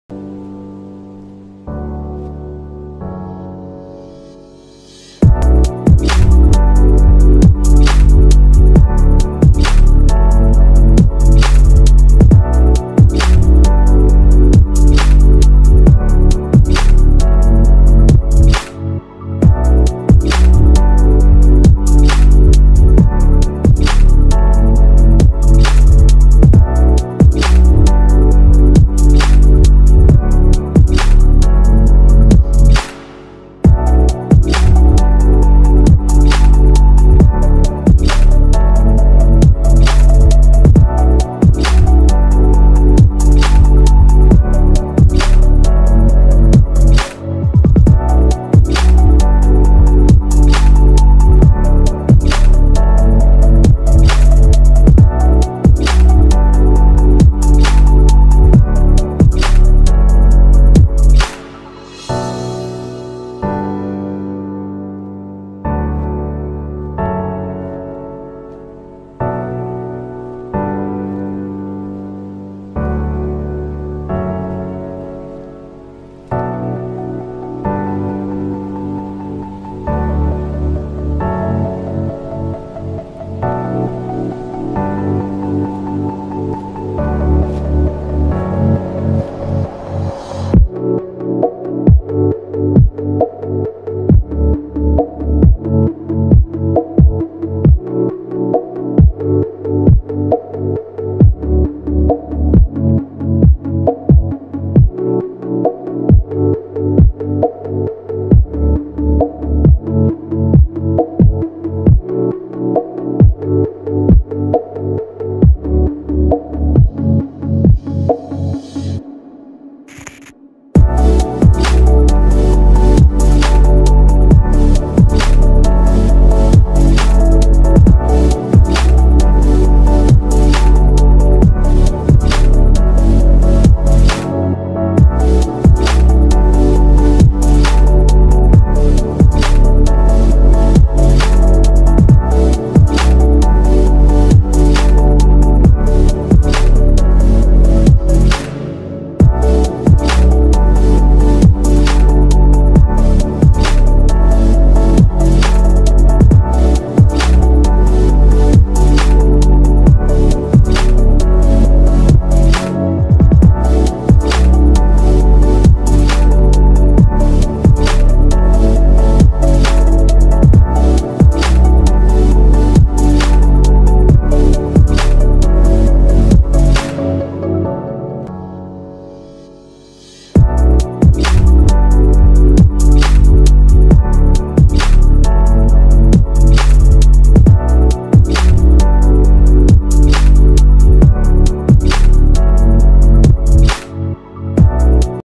DRAWING REALISM WITH ONLY 3 COLORS | Doms colour pencils
Drawing challenge (making a drawing using only 3 colour)

Material used
Brustro grey toned sketchbook
Doms pencil colour (black brown white)
brustro electric eraser
white gel pen
knife for details

Payalkaushikarts